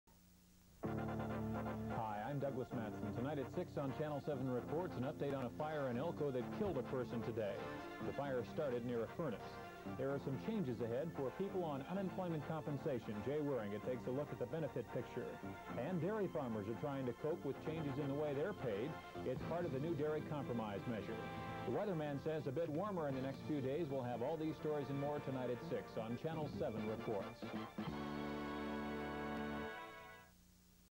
Channel 7 Reports commercial 1983
Commercial from a December 1983 beta recording of WKRP in Cincinnati and the game show The Joker's Wild on WSAW TV 7 Wausau CBS